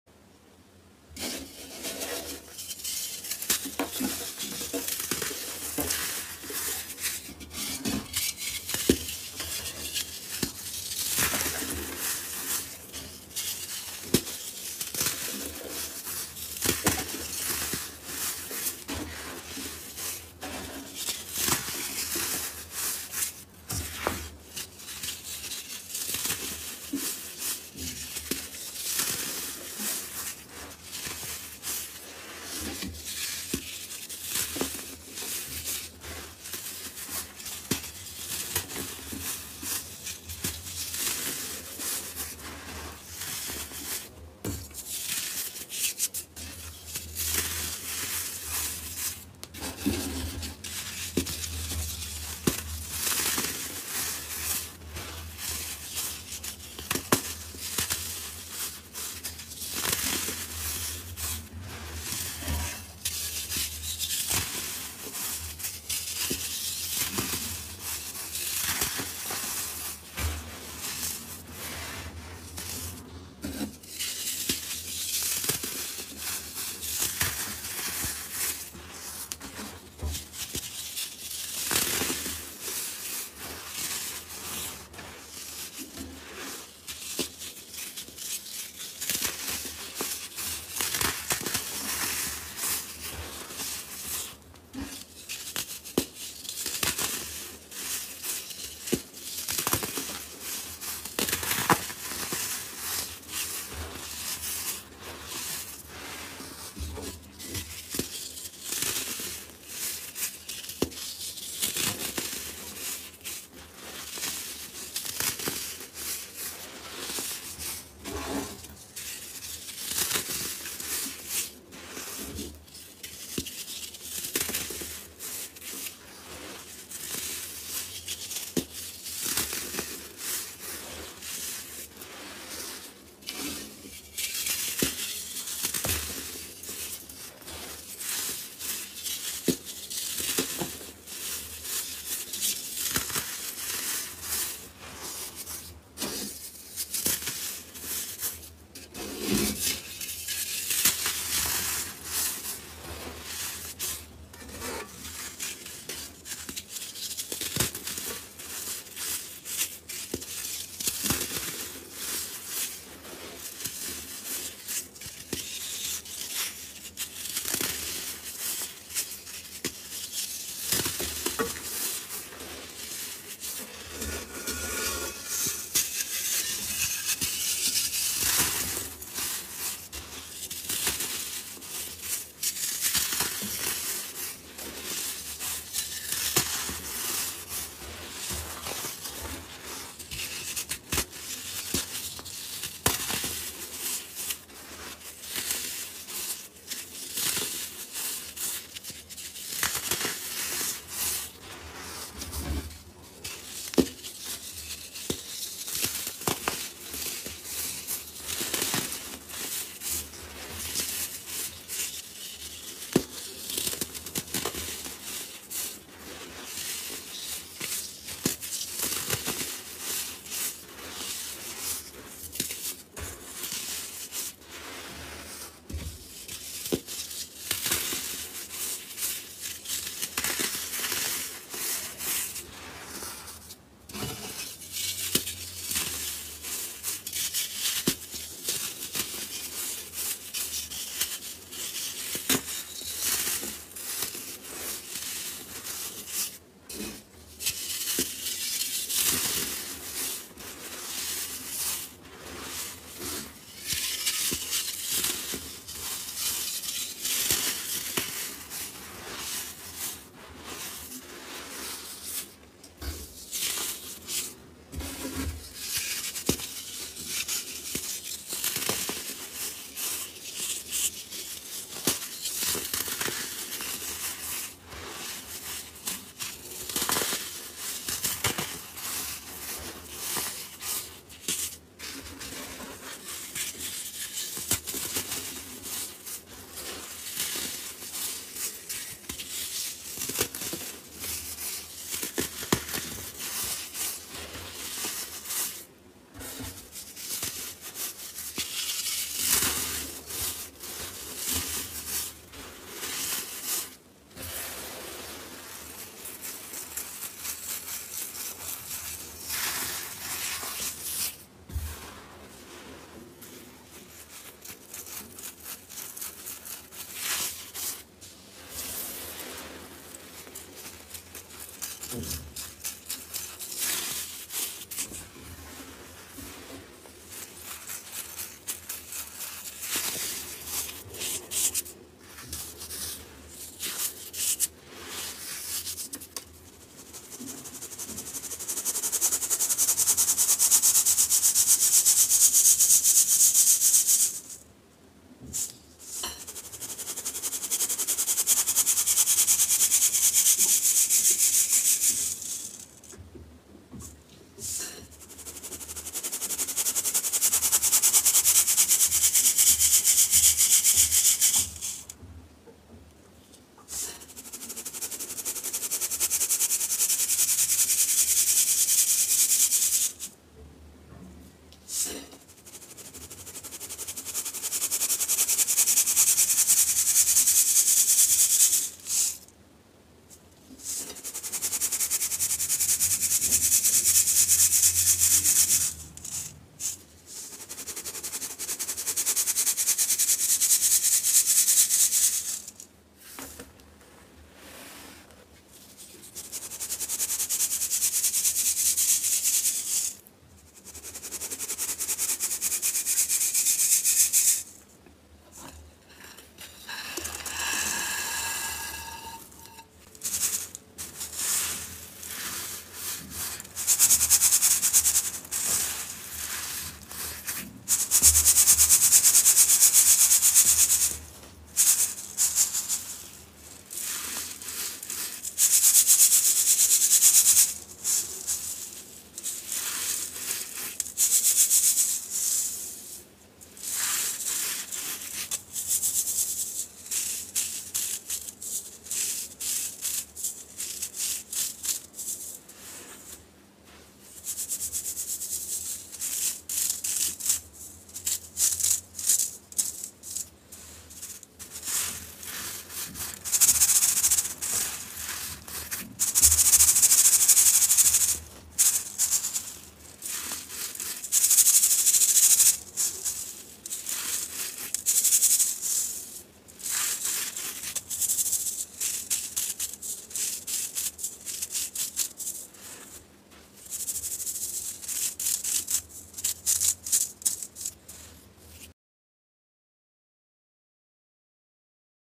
ASMR Baking Soda Monochrome Crush
Hello everyone.
Always looking forward for your likes, feedbacks and comments to improve my work.

00:00 - Crushing
05:35 - Sifting And Collecting Pebbles
06:50 - Crushing Pebbles